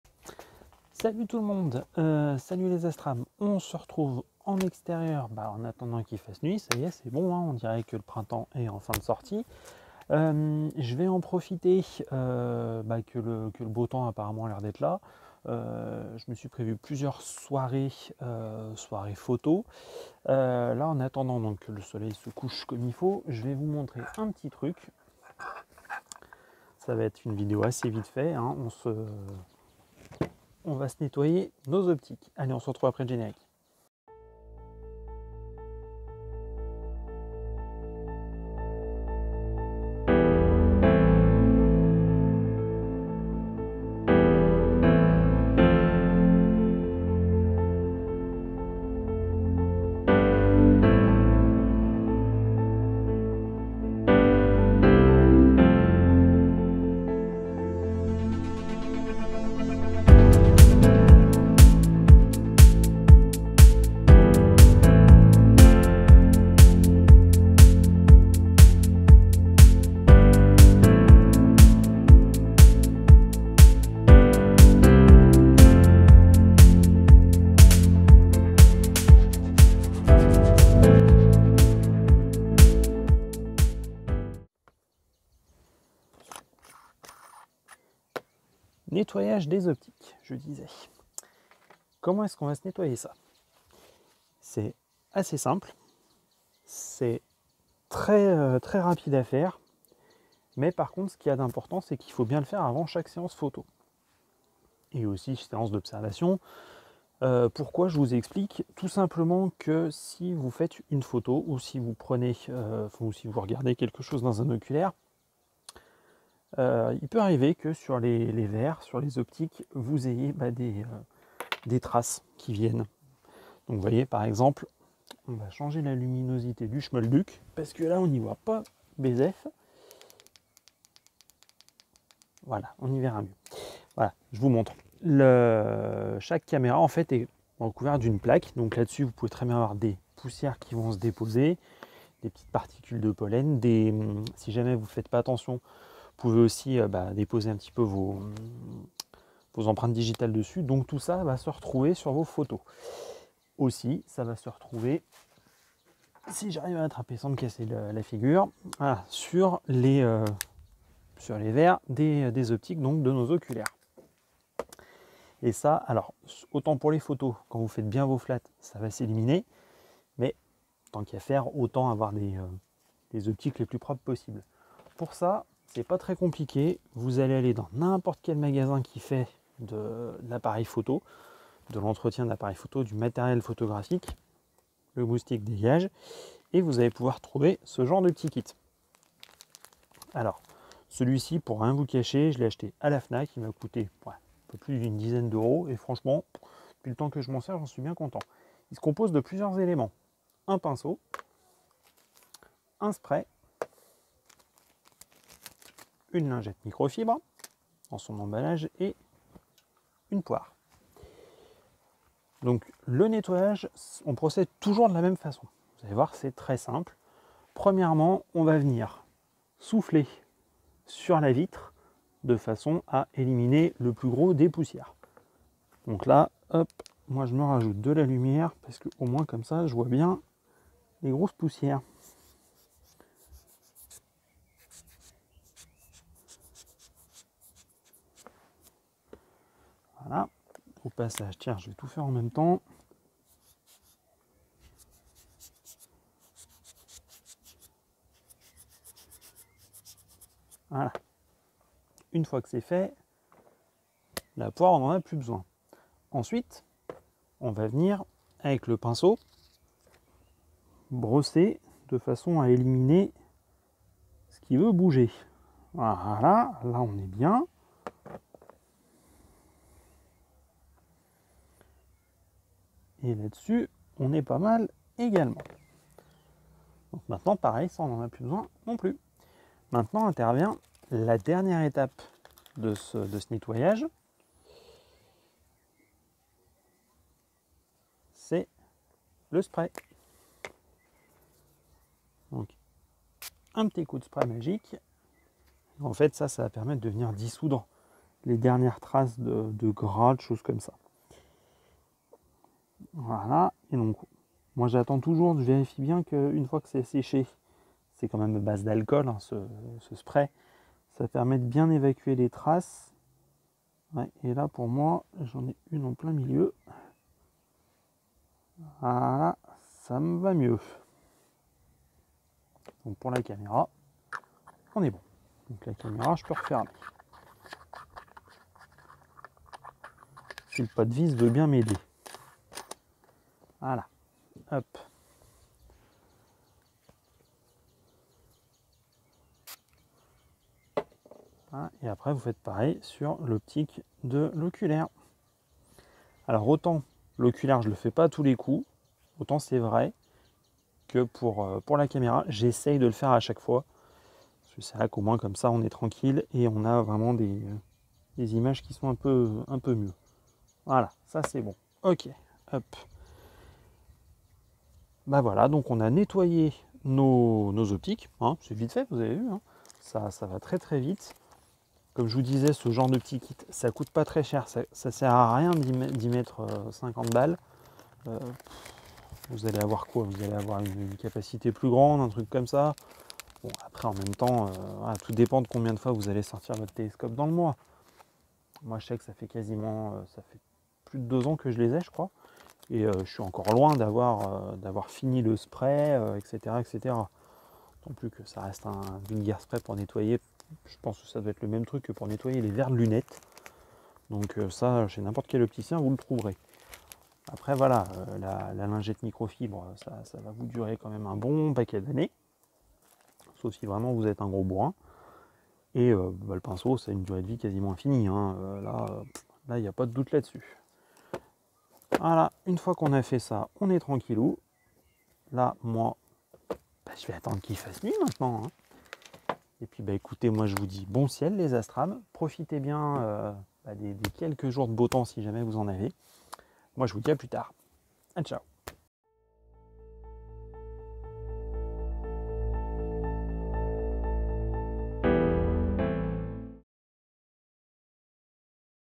Nettoyer ses optiques
🌠🌌Salut les astrams,
dans cette vidéo très courte par rapport aux dernières qui sont sorties sur la chaîne, on va voir comment prendre soin de ses optiques. Que se soit les oculaires ou les caméras, le principe reste le même.
Il faut faire preuve (comme souvent en astro) de rigueur, de méthode et pour cette opération de douceur. Ca ne sert à rien d'appuyer comme un bourrin. On fait du nettoyage en douceur.
Aller on se remonte les manches et on y va.
A tchao les astrams.😉